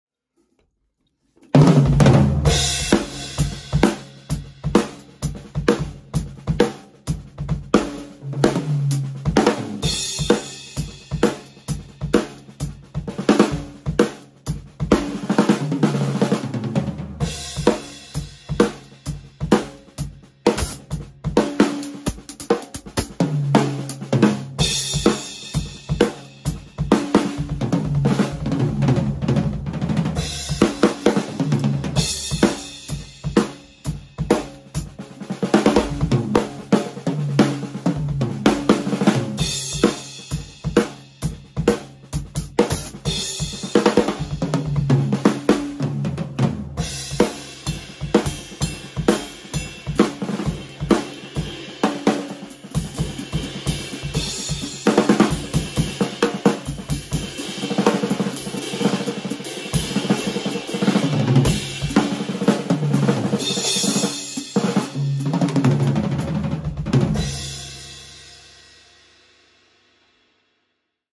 Orlando Hernandez Jr. Having a little fun today!
Having fun today with a little solo, only iPhone used, no processing at all, drum sound into iPhone, I hope you guys like it!!